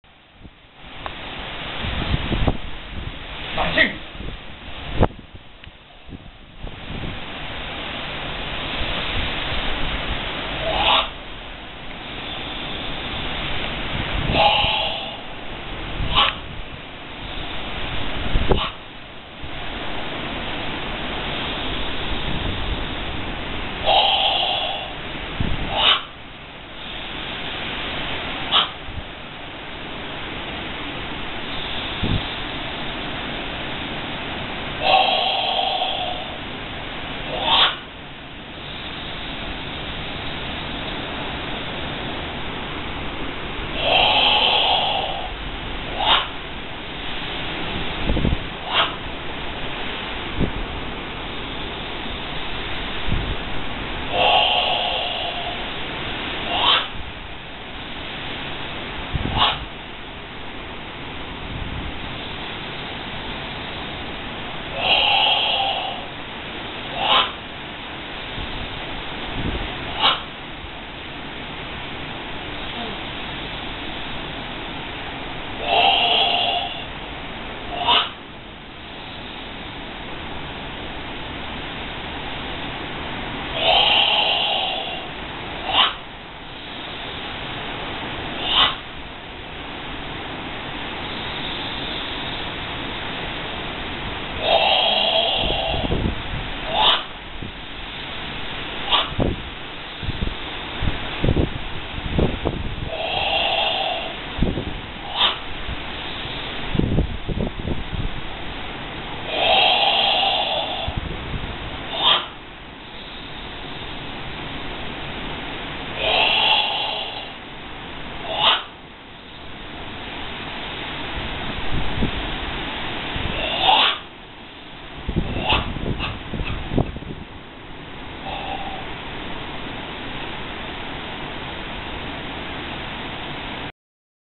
Goju-ryu. Sanchin.
Higaonna Kanryo Sensei Sanchin during the typhoon in Okinawa